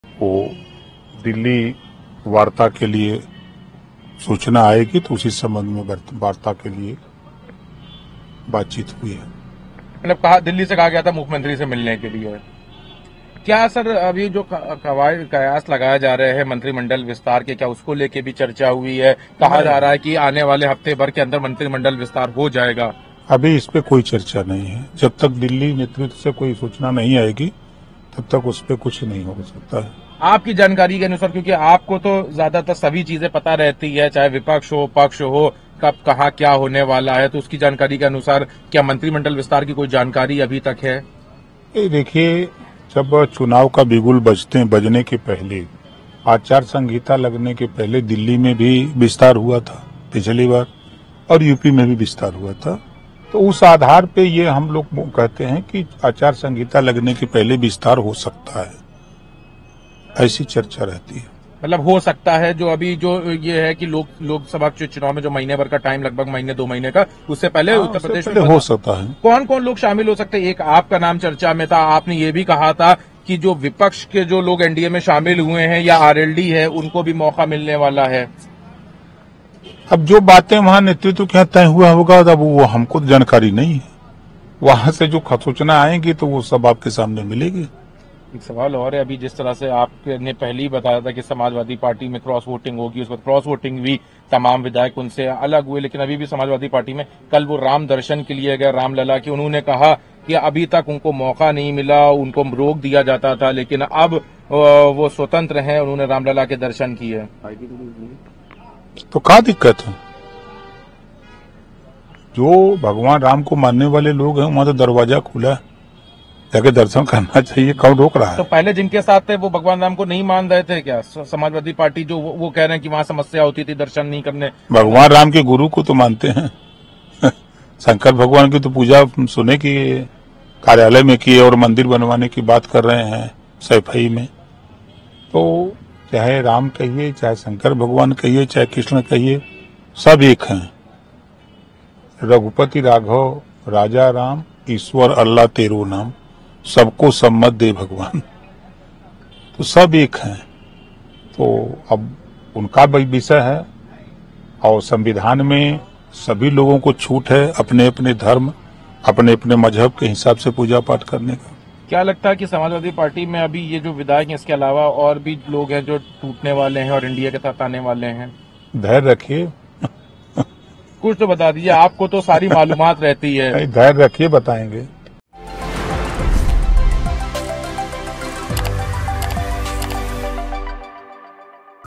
OP Rajbhar on UP Cabinet Expansion: CM Yogi के मंत्रिमंडल विस्तार पर क्या बोले राजभर | NBT
लंबे समय से योगी आदित्यनाथ मंत्रिमंडल के विस्तार की अटकलें लगाई जा रही हैं। बताया जा रहा है कि लोकसभा चुनाव से पहले 3 मार्च तक यूपी सरकार में कुछ नए मंत्री शामिल हो सकते हैं। सुभासुपा प्रमुख ओमप्रकाश राजभर, सपा से बीजेपी में वापस आए दारा सिंह चौहान सहित राष्‍ट्रीय लोकदल के एक विधायक को मंत्री बनाया जा सकता है। खबरों पर ओपी राजभर ने अपनी प्रतिक्रिया दी है ।